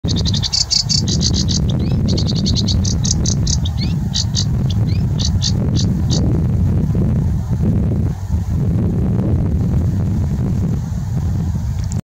Sedge Warbler Singing
Sedge Warbler singing at National Trust Croome